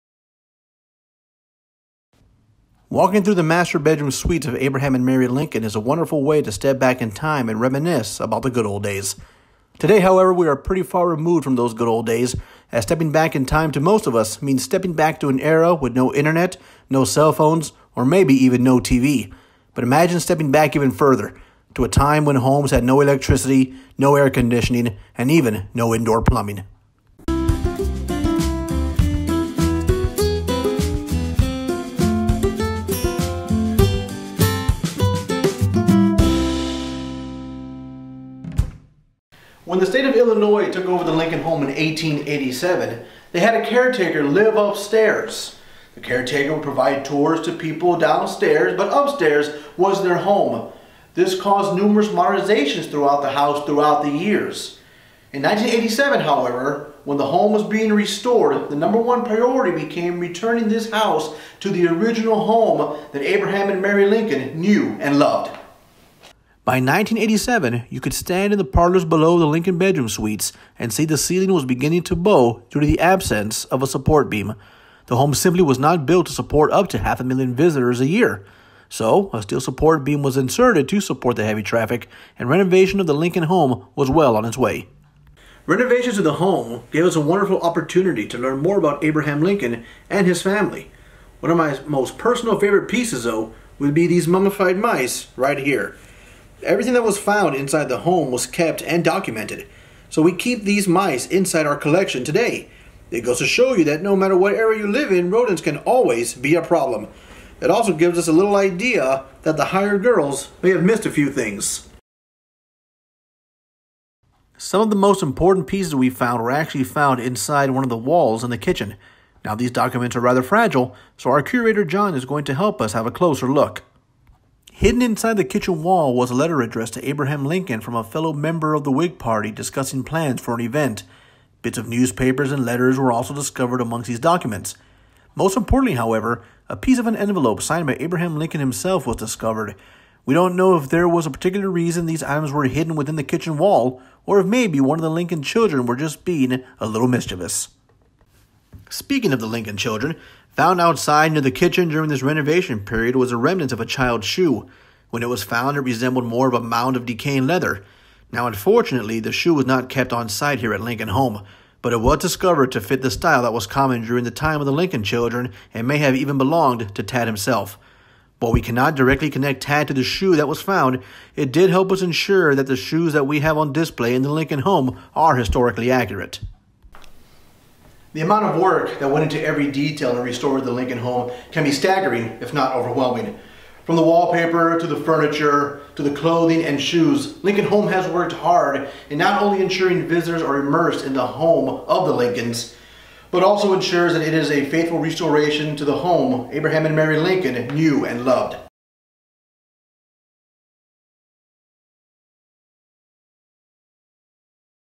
At Home with the Lincolns Ep. 16: Discoveries in Restoration
Do you believe "Home is where the Heart is?" Abraham and Mary Lincoln believed so, as they cherished their home in Springfield, IL. However, the years after they moved out would bring many changes to the home they knew and held so dearly. This week, join Ranger Paul in his final "At Home with the Lincolns" video as he explores letters and papers that were found hidden inside one of the walls in the Lincoln Home kitchen, to the mummified remains of some early inhabitants of the Lincoln home! It as an in depth look into the amount of work that Lincoln Home National Historic Site faced in faithfully restoring the original  home that Abraham and Mary Lincoln knew and loved.